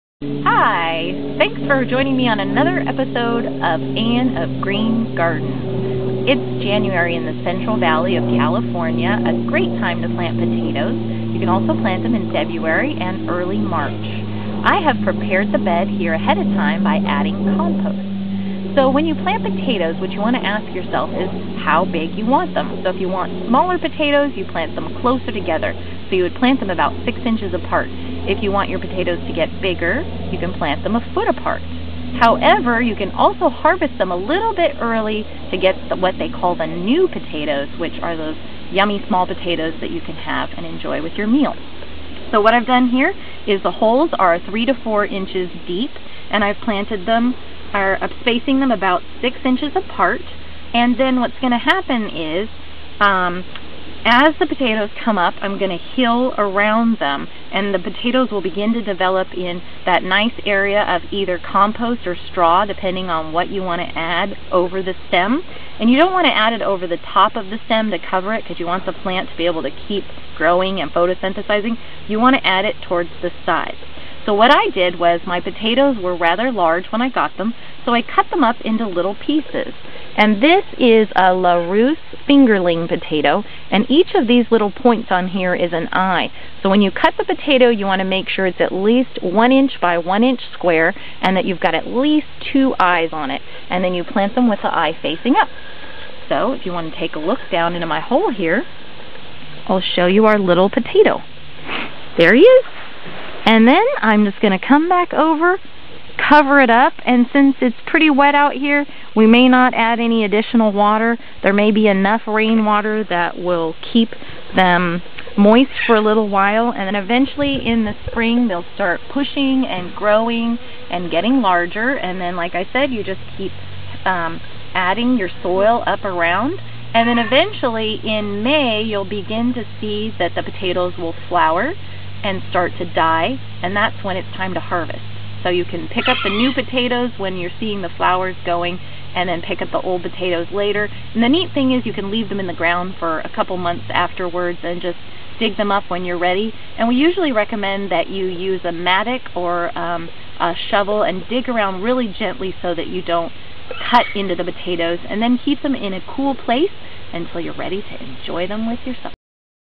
How to Plant Potatoes
Anne shows how to plant potatoes in your garden.